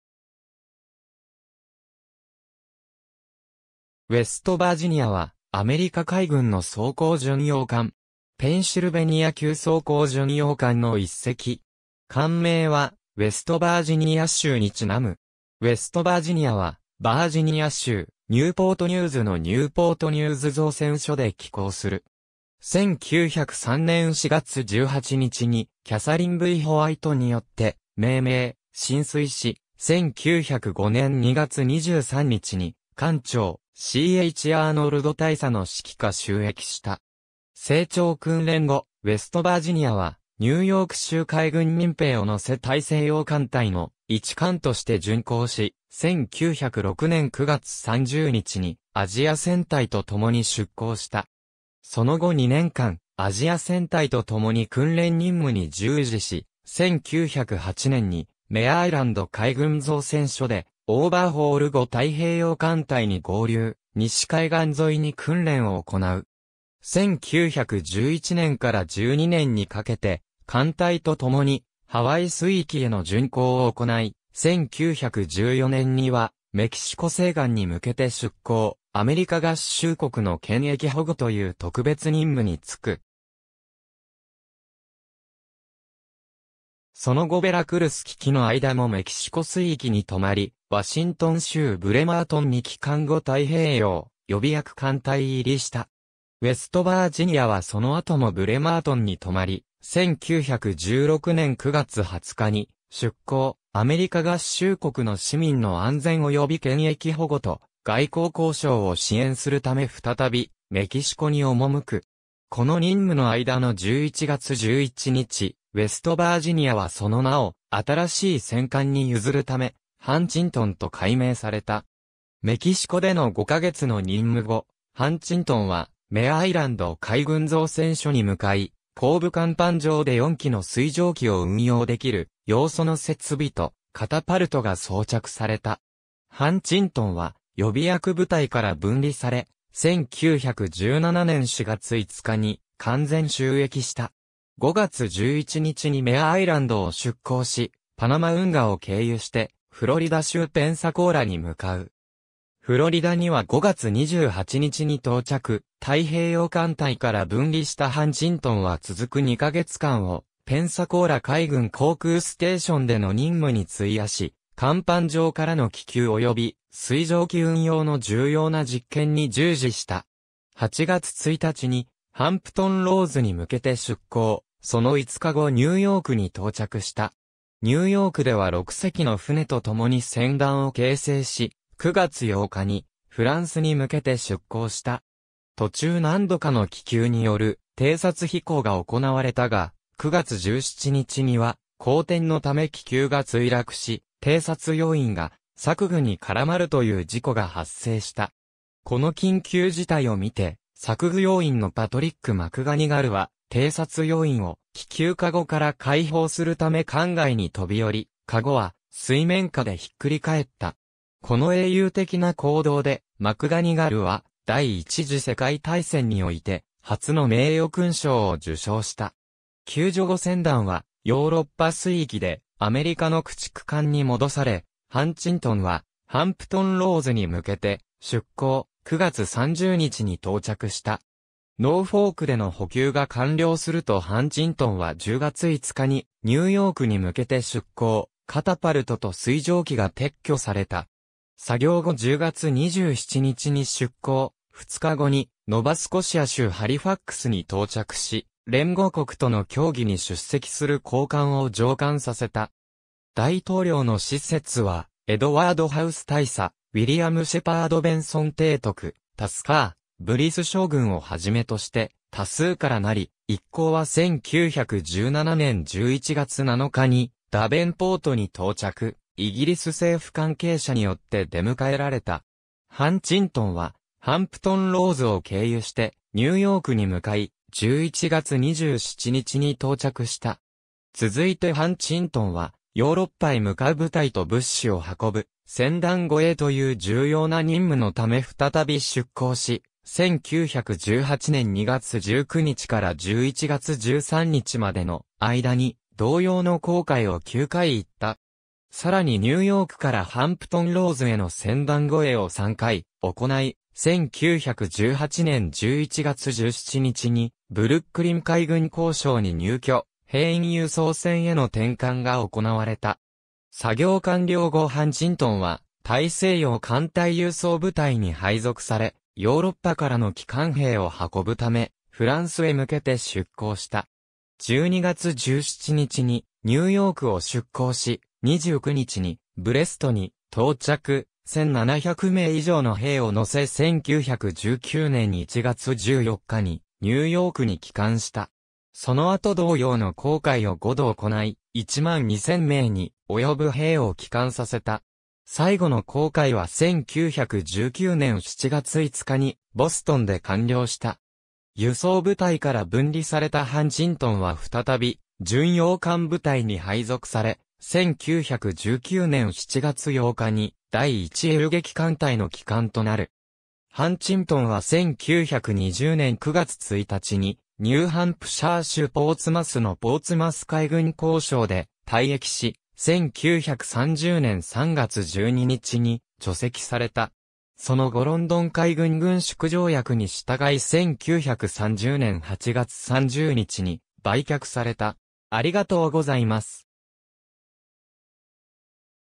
ウェストバージニア (装甲巡洋艦)
ウェストバージニア（USS West Virginia, ACR-5 ）は、アメリカ海軍の装甲巡洋艦。
ペンシルベニア級装甲巡洋艦の一隻。
艦名はウェストバージニア州に因む。
ウェストバージニアはバージニア州ニューポート・ニューズのニューポート・ニューズ造船所で起工する。
1903年4月18日にキャサリン・V・ホワイトによって命名、進水し、1905年2月23日に艦長C・H・アーノルド大佐の指揮下就役した。
整調訓練後、ウェストバージニアはニューヨーク州海軍民兵を乗せ大西洋艦隊の一艦として巡航し、1906年9月30日にアジア戦隊と共に出航した。
その後2年間、アジア戦隊と共に訓練任務に従事し、1908年にメア・アイランド海軍造船所でオーバーホール後太平洋艦隊に合流、西海岸沿いに訓練を行う。
1911年から12年にかけて艦隊とともにハワイ水域への巡航を行い、1914年にはメキシコ西岸に向けて出航、アメリカ合衆国の権益保護という特別任務に就く。
その後ベラクルス危機の間もメキシコ水域に留まり、ワシントン州ブレマートンに帰還後太平洋予備役艦隊入りした。
ウェストバージニアはその後もブレマートンに留まり、1916年9月20日に出航、アメリカ合衆国の市民の安全および権益保護と、外交交渉を支援するため再びメキシコに赴く。
この任務の間の11月11日、ウェストバージニアはその名を新しい戦艦に譲るため、ハンチントン (USS Huntington, CA-5) と改名された。
メキシコでの5ヶ月の任務後、ハンチントンはメア・アイランド海軍造船所に向かい、後部甲板上で4機の水上機を運用できる様その設備とカタパルトが装着された。
ハンチントンは予備役部隊から分離され、1917年4月5日に完全就役した。
5月11日にメア・アイランドを出航しパナマ運河を経由してフロリダ州ペンサコーラに向かう。
フロリダには5月28日に到着、太平洋艦隊から分離したハンチントンは続く2ヶ月間をペンサコーラ海軍航空ステーションでの任務に費やし、甲板上からの気球および水上機運用の重要な実験に従事した。
8月1日にハンプトン・ローズに向けて出航、その5日後ニューヨークに到着した。
ニューヨークでは6隻の船と共に船団を形成し、9月8日にフランスに向けて出航した。
途中何度かの気球による偵察飛行が行われたが、9月17日には荒天のため気球が墜落し、偵察要員が索具に絡まるという事故が発生した。
この緊急事態を見て索具要員のパトリック・マクガニガルは偵察要員を気球篭から解放するため艦外に飛び降り、篭は水面下でひっくり返った。
この英雄的な行動でマクガニガルは第一次世界大戦において初の名誉勲章を受章した。
救助後船団はヨーロッパ水域でアメリカの駆逐艦に戻され、ハンチントンはハンプトン・ローズに向けて出航、9月30日に到着した。
ノーフォークでの補給が完了するとハンチントンは10月5日にニューヨークに向けて出航、カタパルトと水上機が撤去された。
作業後10月27日に出航、2日後にノバスコシア州ハリファックスに到着し、連合国との協議に出席する高官を乗艦させた。
大統領の使節はエドワード・ハウス大佐、ウィリアム・シェパード・ベンソン提督、タスカー・ブリス将軍を始めとして多数から成り、一行は1917年11月7日にダベンポートに到着、イギリス政府関係者によって出迎えられた。
ハンチントンはハンプトン・ローズを経由してニューヨークに向かい、11月27日に到着した。
続いてハンチントンはヨーロッパへ向かう部隊と物資を運ぶ船団護衛という重要な任務のため再び出航し、1918年2月19日から11月13日までの間に同様の航海を9回行った。
さらにニューヨークからハンプトン・ローズへの船団護衛を3回行い、1918年11月17日にブルックリン海軍工廠に入渠、兵員輸送船への転換が行われた。
作業完了後ハンチントンは大西洋艦隊輸送部隊に配属され、ヨーロッパからの帰還兵を運ぶためフランスへ向けて出航した。
12月17日にニューヨークを出航し、29日にブレストに到着、1,700名以上の兵を乗せ1919年1月14日にニューヨークに帰還した。
その後同様の航海を5度行い、12,000名に及ぶ兵を帰還させた。
最後の航海は1919年7月5日にボストンで完了した。
輸送部隊から分離されたハンチントンは再び巡洋艦部隊に配属され、1919年7月8日に第1遊撃艦隊の旗艦となる。
ハンチントンは1920年9月...